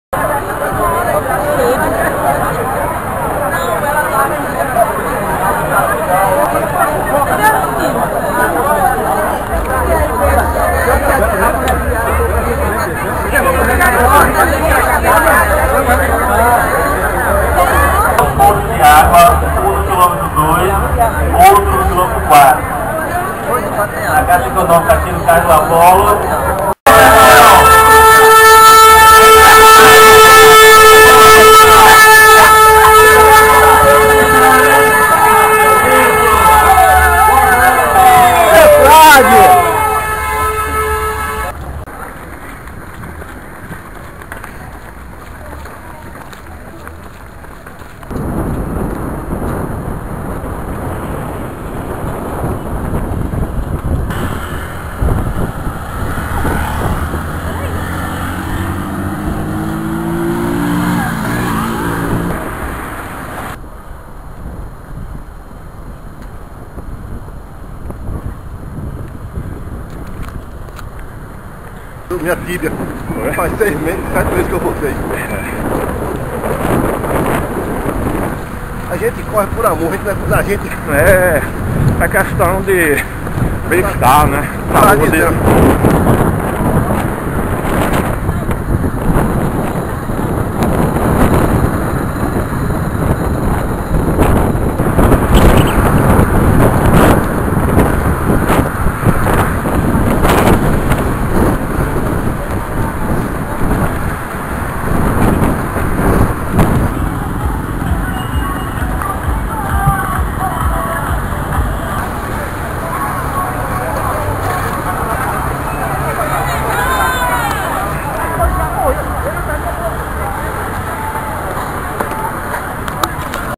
Corrida Caixa
Registro da participação de Marcos V. Carvalho na Corrida do pessoal da Caixa, realizada no dia 17/05/14 em Recife. Vídeo: Marcos V. Carvalho